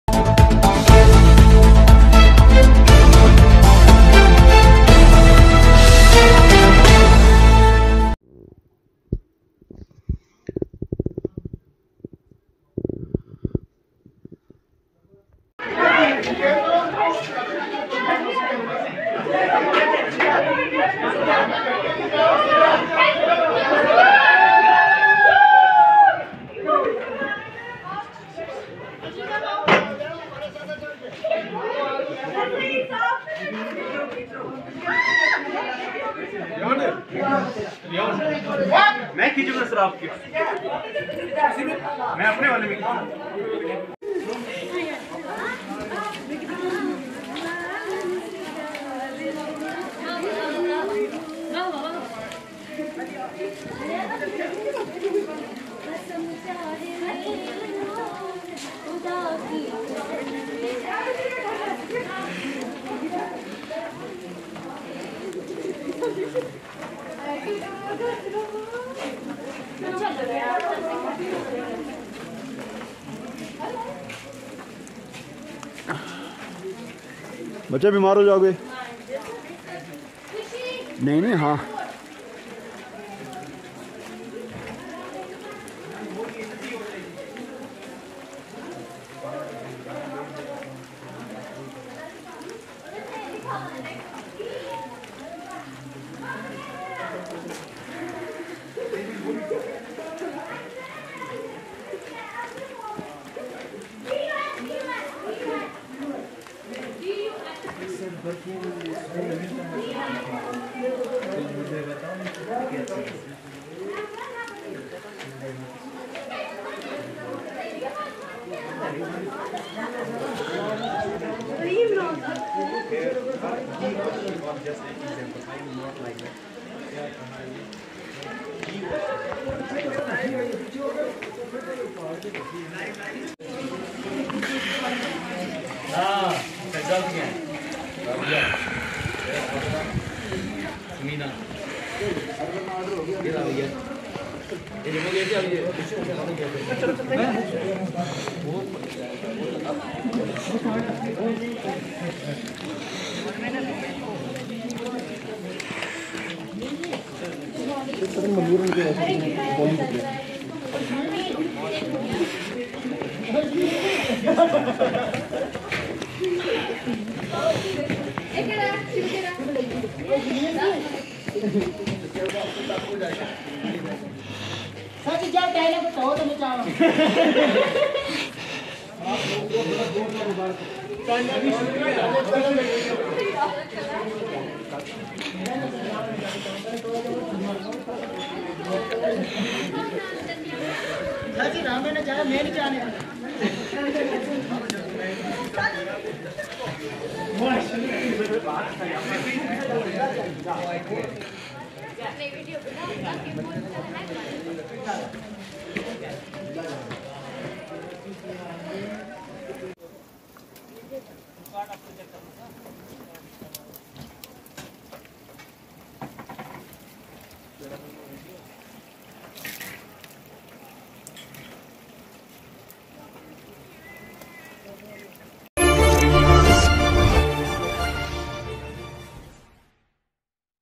Seasonal First Fresh Snowfall At Sawjiya, Most Beautiful Place Sawjiya Mandi, People Having Fun In T
Seasonal First Fresh Snofall At Sawjiya, Most Beautiful Place Sawjiya Mandi, People Having Fun In The Snowfall